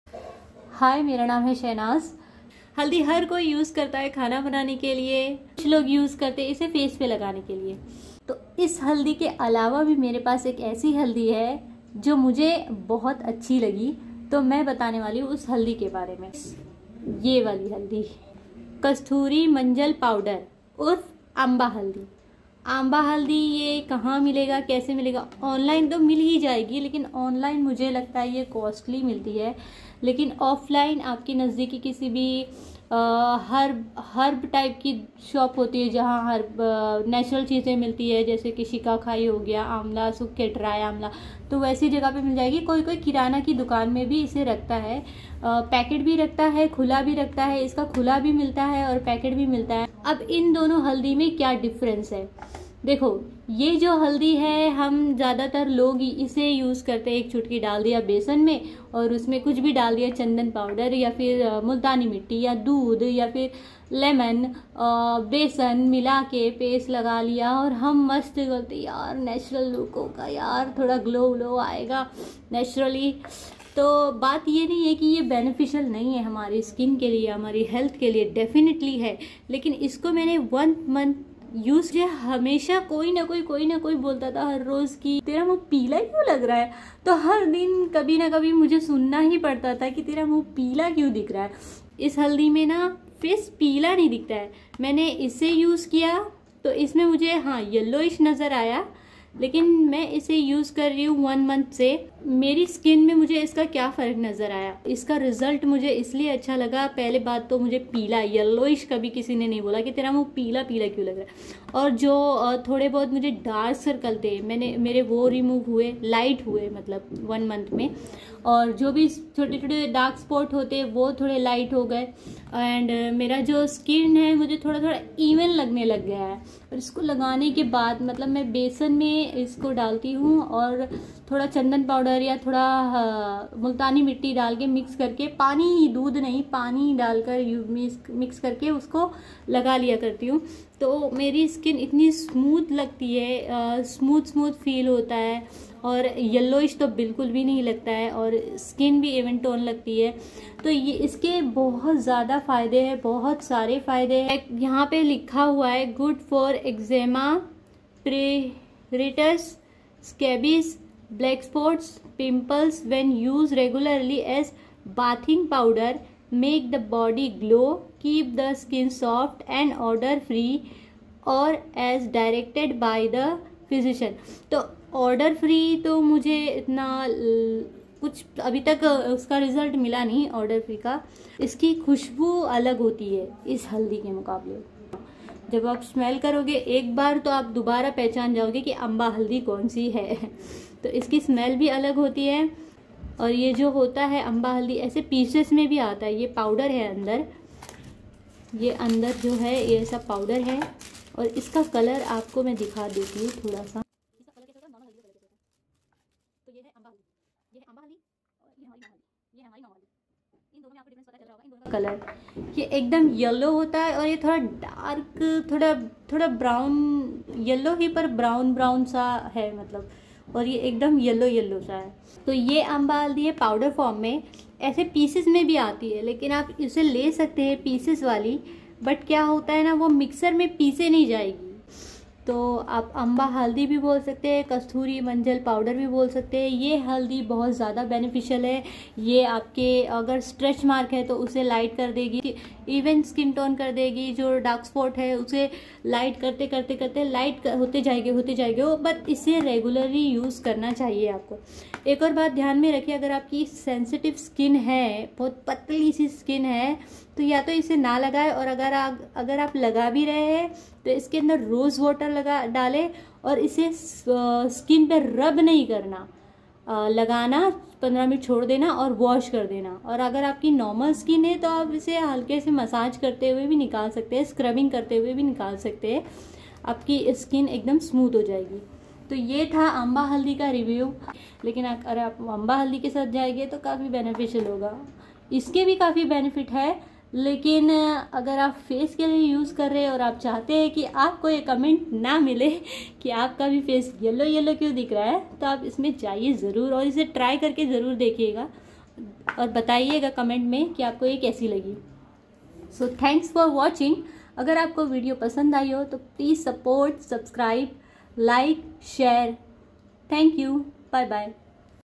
चेहरे पर पीली हल्दी लगाने से पहले देखें ये वीडियो | Ammba Haldi Review
Hi,
Maira Naam Hai Shahnaz
About
Ammba Haldi / kasthuri manjal powder
Review

 Uptan
1 tablespoon behan
Half tablespoon ammba haldi
2 pinch Chandan powder
Half tablespoon Multani mitti
Water / rose water
mix it well and apply on your face and neck
wash it out after 15-20 mints